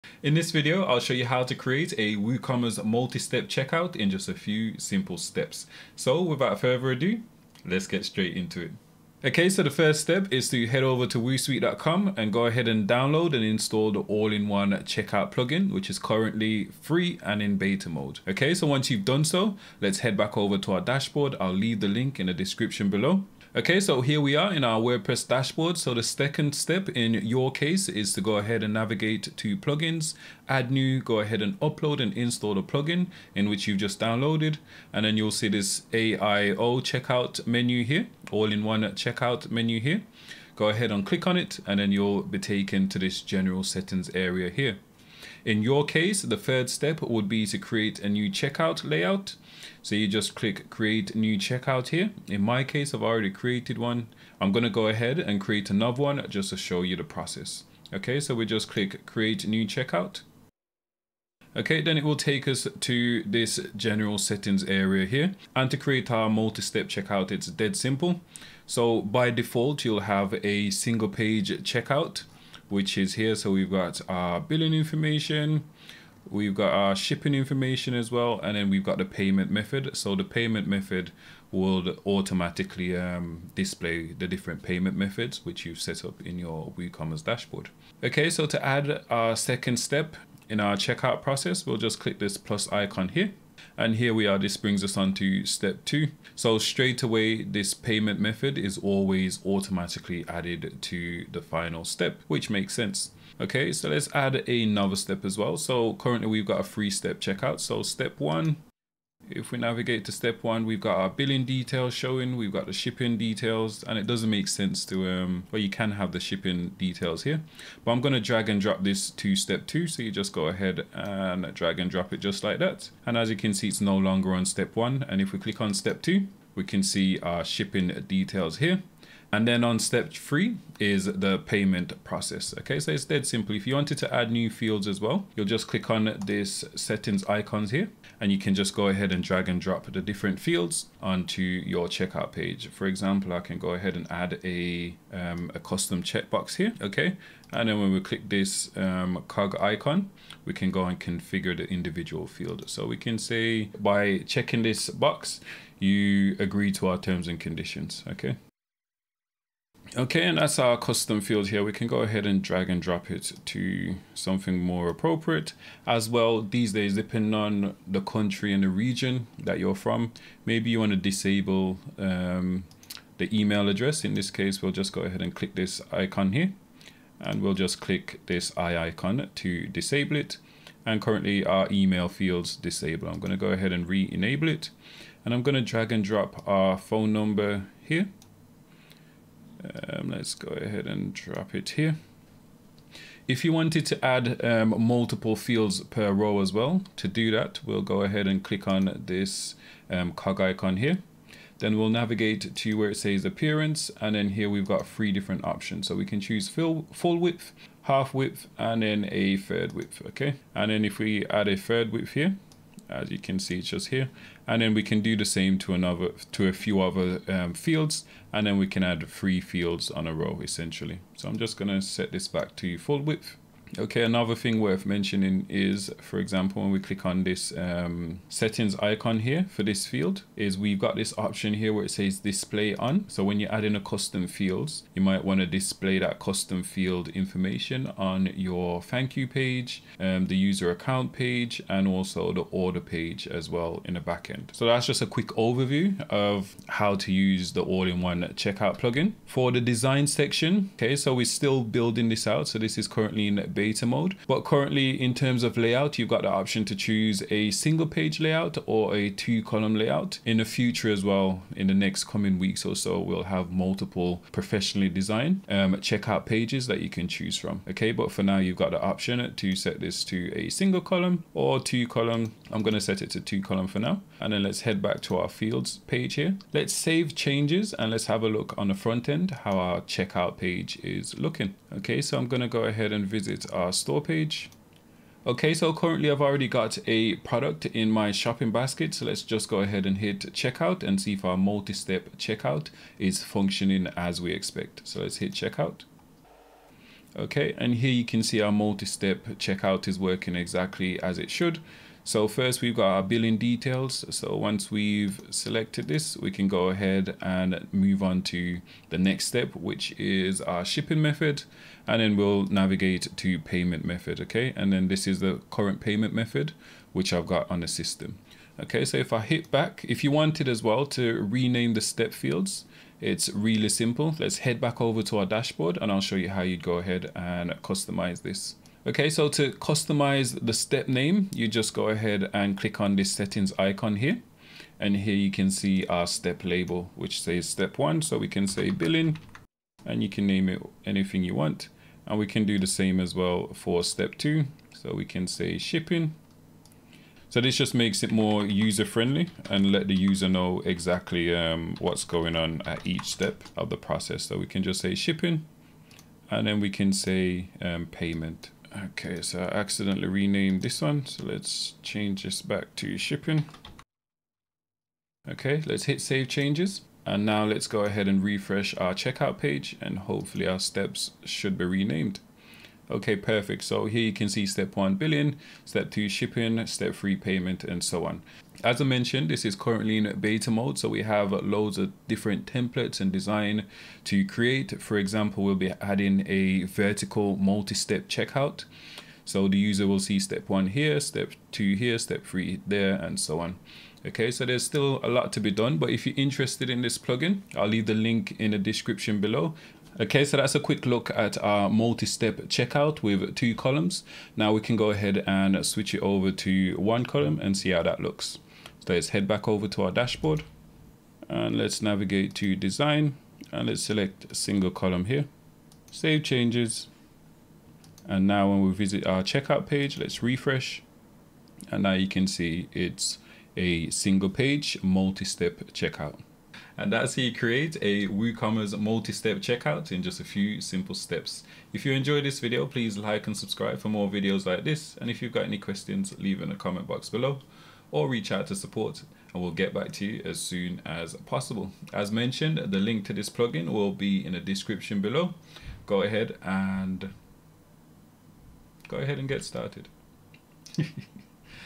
Free WooCommerce MultiStep Checkout (& Checkout Field Customisation
In this video, I'll show you how to create a  Free WooCommerce multistep checkout in just a few simple steps.

00:00 intro
00:09 Download All in One Checkout
00:25 Upload add activate All in One Checkout
00:45 Create a new Checkout
01:05 Create Multi Step Checkout
02:15 Custom Checkout Field
04:57 Preview Multi Step Checkout
05:22 Rename Checkout Steps
06:44 How to enable Multi Step Checkout 1 Column
07:06 Conclusion